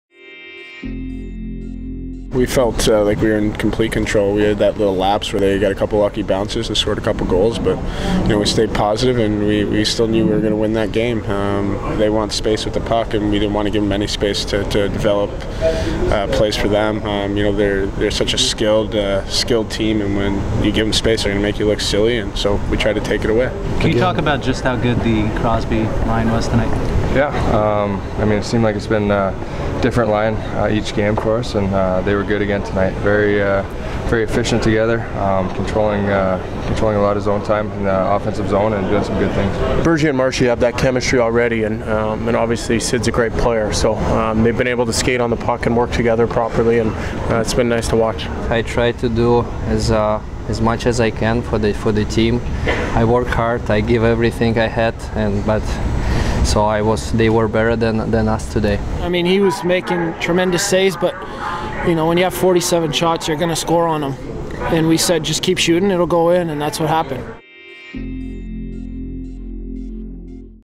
Canada felt in 'complete control' defeating Russia: Doughty
Canada defeated Russia 5-3 in the World Cup of Hockey semifinal Saturday night. Defenceman Drew Doughty says the team was focused on not giving Russia any space with the puck. (September 25)